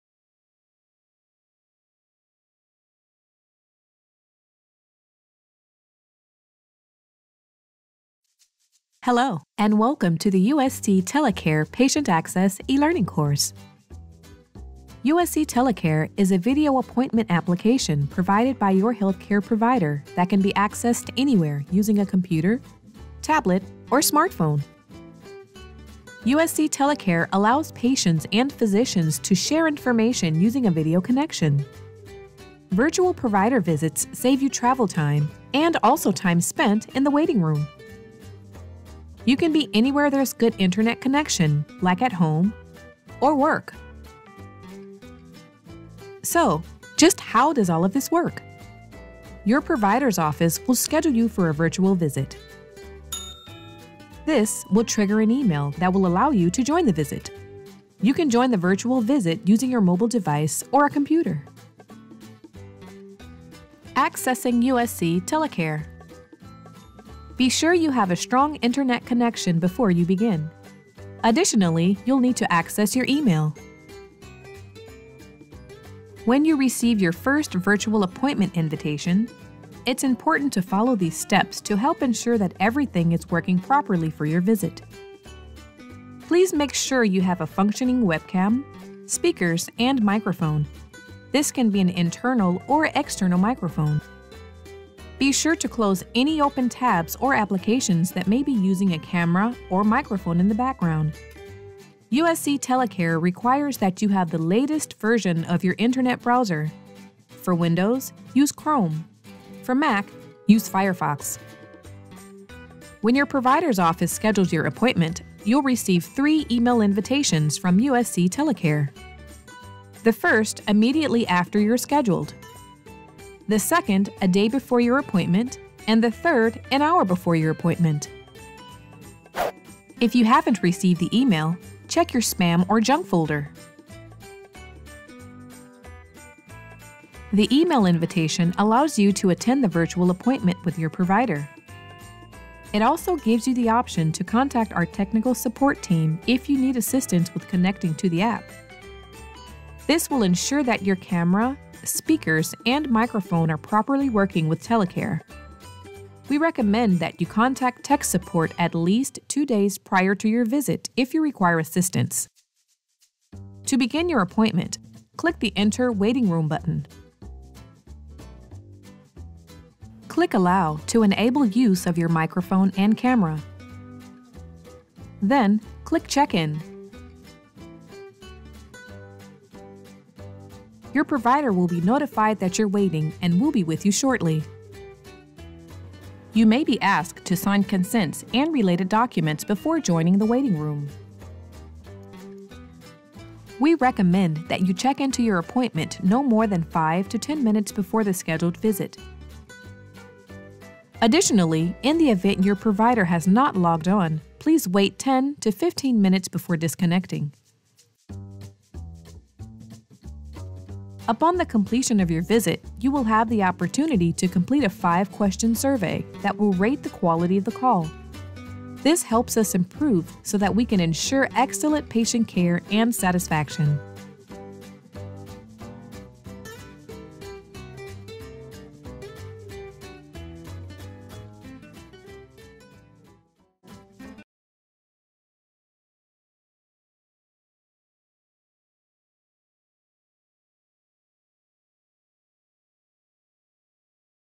How to Install USC TeleCARE on Your Desktop Computer
This training guide provides instructions on how to install Keck Medicine of USC's TeleCARE platform on your desktop computer.